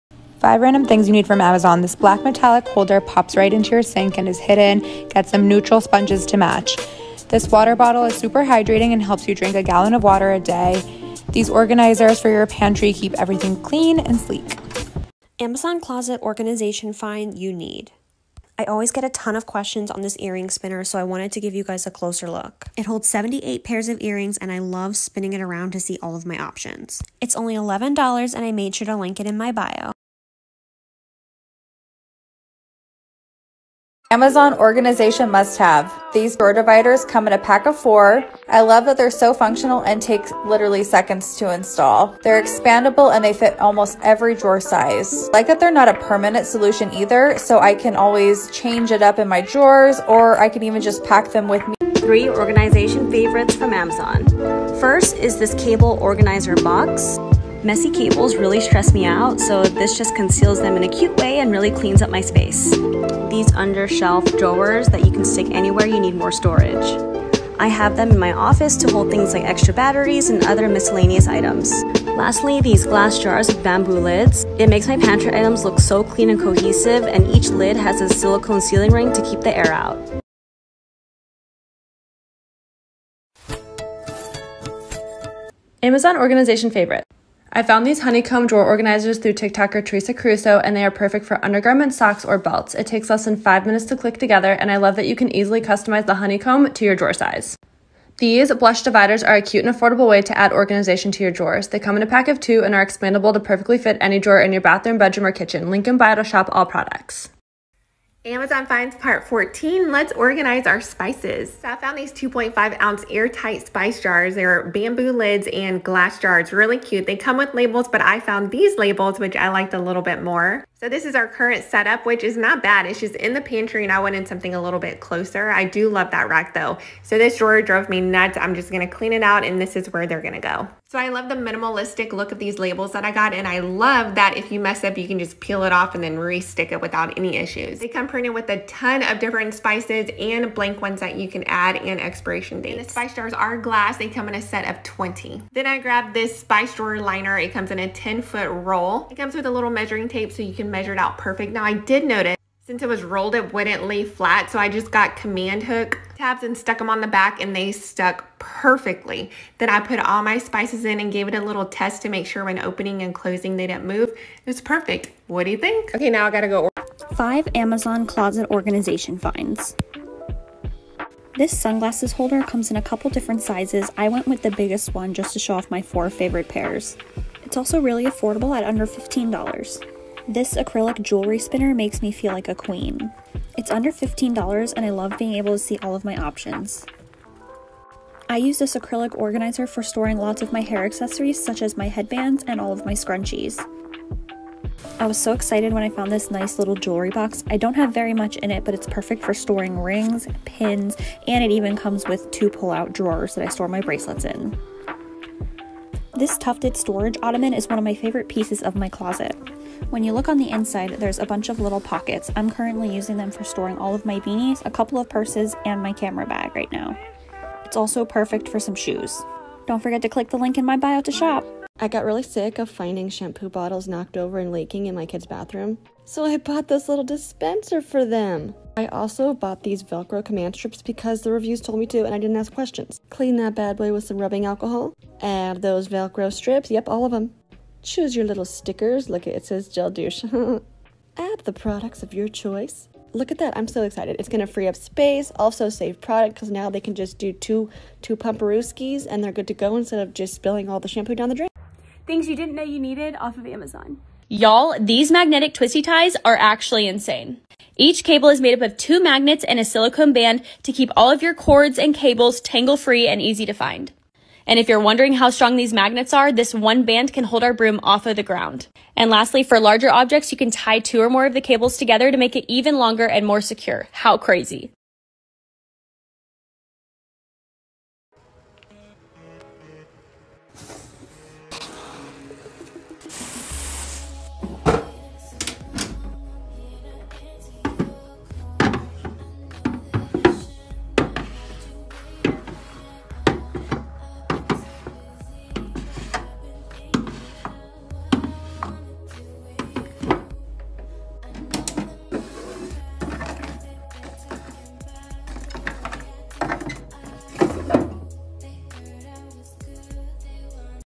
TIKTOK ORGANIZATION HACKS AMAZON FINDS 2021 AMAZON MUST HAVES WITH LINKS
Hello everyone! I am back with the best organization hacks!
Am guesssing it's not just me but i feel more productive when my space is tidy.  it is not always easy, as you might want not have enough space for items and want to save as much space as possible.
I love finding little tricks to get my home organized whilst still saving space and today I wanted to share the best ones with you! I found some of the items on Amazon and linked them down below. I hope you like it!
Have a lovely week!

Video #1 @inourcart
black metallic wash holder
water bottle
pantry organiser

Video #2 @serenaajoyce
rotating earring holder organizer

Video #3 @dawnmoore
wardrobe divider

Video #4 @bymichellelei
cable organiser box
under shelf drawers
glass container with bamboo lids

Video #5 @juliana_claire
honeycomb draw organisers
draw dividers

Video #6 @jacquelineaniece
spice jars with bamboo lids
spice draw liners

Video #7 @serenajoyce
sunglasses holder
Acrylic jewllery spinner
Acrylic organiser
Jewellery box

Video #8 @busybeingkaylee
shampoo dispenser
Command strip

Video #9 @rachel_meaders
magnetic twisty ties

Video #10 @kaelmace
Skincare storage